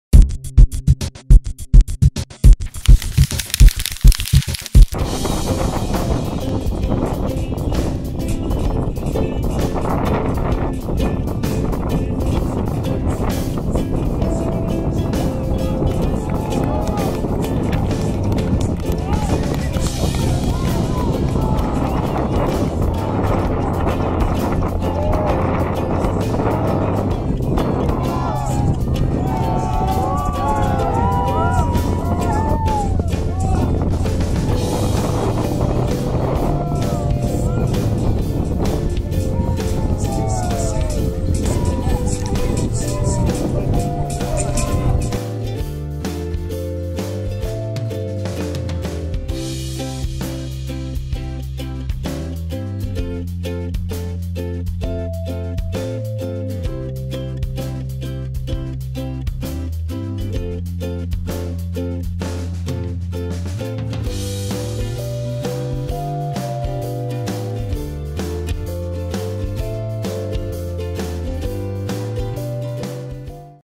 Cabra Corral: Puenting y Bungee Jumping
Dique Cabra Corral - Saltos desde el puente.